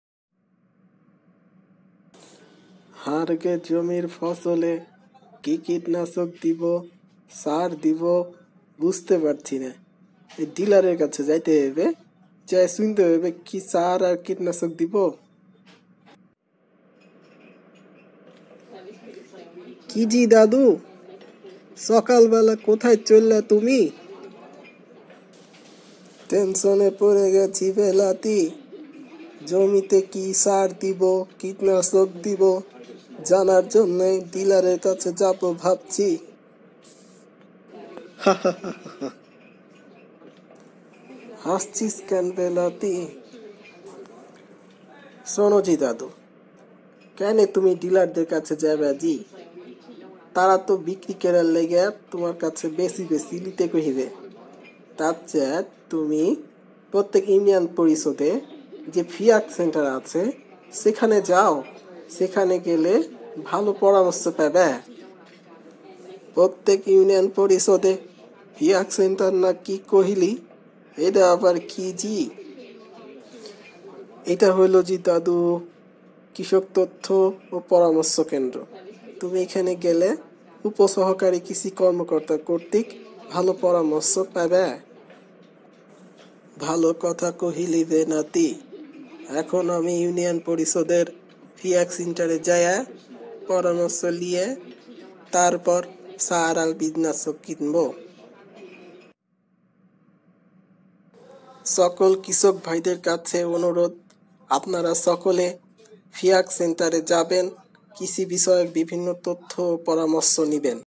কৃষক ভাইরা কোথায় যাবে????
কৃষক ভাইরা কৃষি বিষয়ক বিভিন্ন তথ্যর জন্য ইউনিয়ন পরিষদের ফিয়াক সেন্টারে যাবে। ফিয়াক সেন্টারে উপসহকারী কৃষি কর্মকর্তারা অফিস সময়ে অবস্থান করেন, তারা পরামর্শ প্রদান করে কৃষকদের সহযোগিতা করবেন। ফিয়াক সেন্টার হচ্ছে কৃষি সম্প্রসারণ অধিদপ্তরের এনএটিপি প্রকল্পের উন্নত উদ্ভাবনী চিন্তার একটি ফসল। ফিয়াক সেন্টারকে কৃষক ভাইদের কাছে পরিচিত করে দেওয়ার উদ্দেশ্যই  কার্টুনটি প্রস্তুত করা হয়েছে।